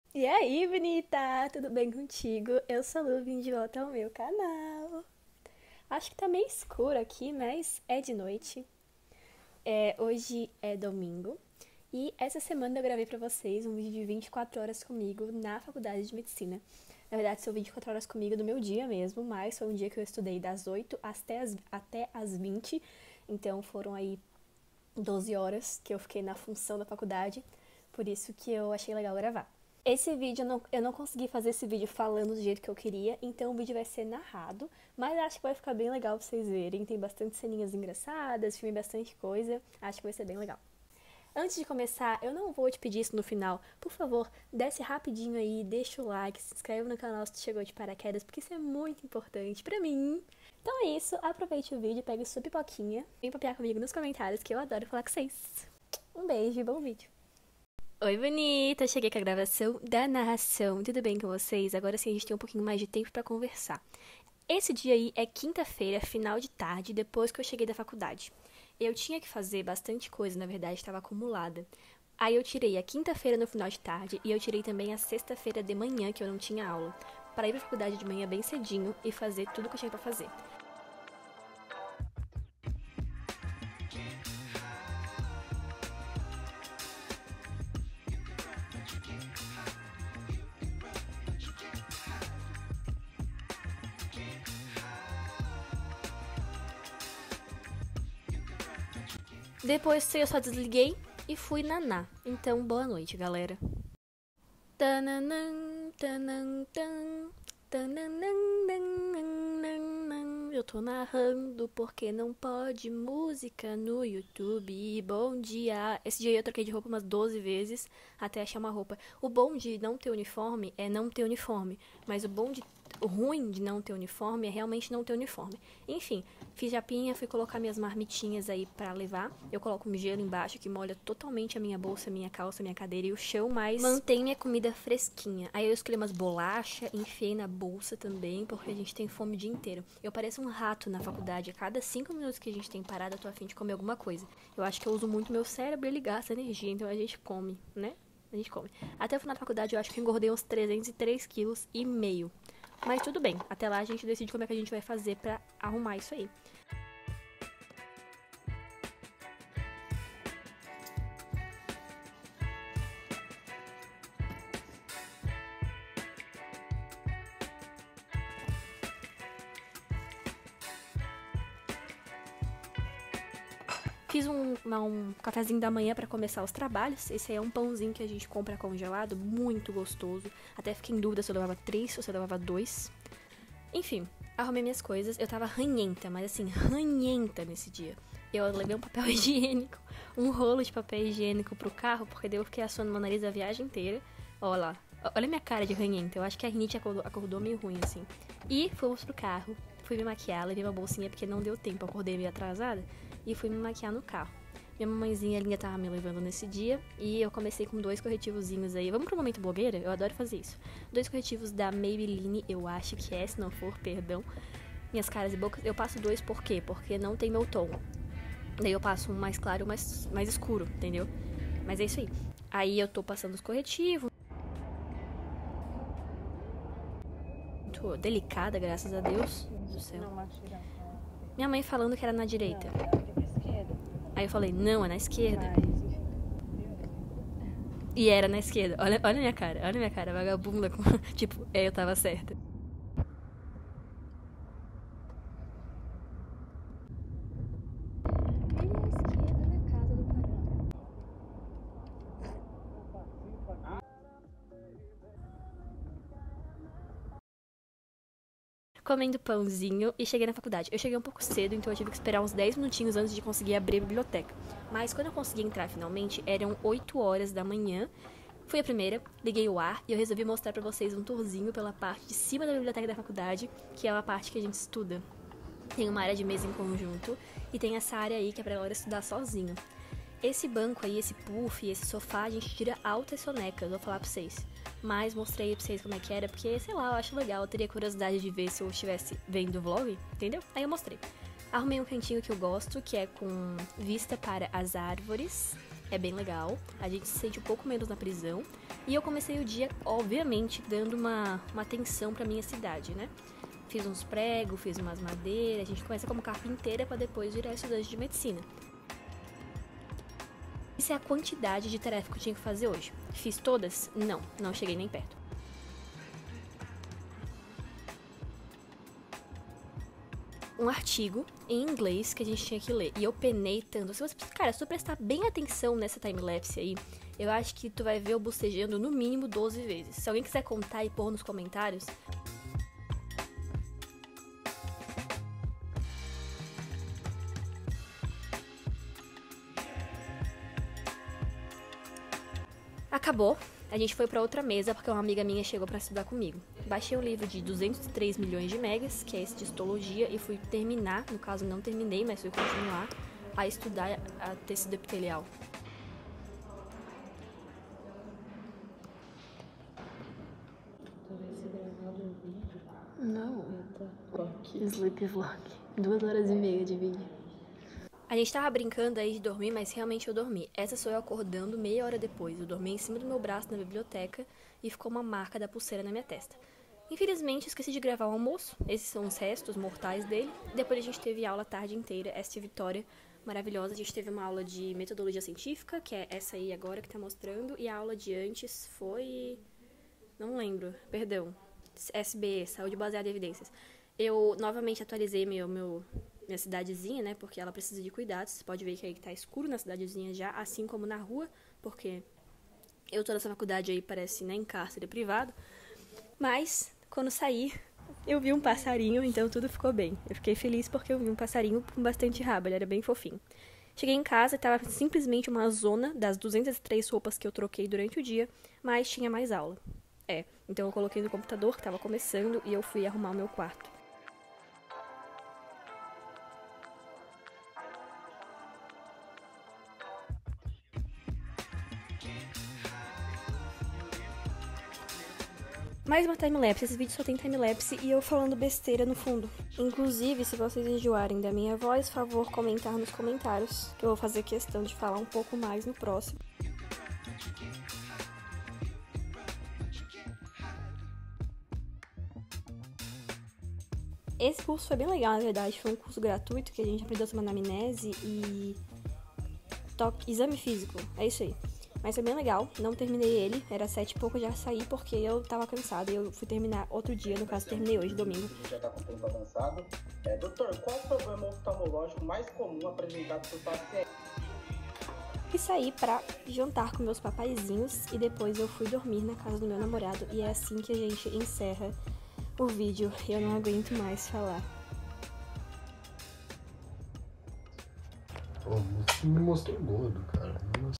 24 Horas de uma Estudante de Medicina | medvlog 2 👩🏻‍⚕️📖
Oii bonita! Gravei um dia inteiro para mostrar a minha rotina na faculdade de medicina.

Mostrei estudos, aulas e pequenos surtos durante o dia hahaha

Vem passar essas 24 horas comigo!

Beijinhos 🤍🤍🤍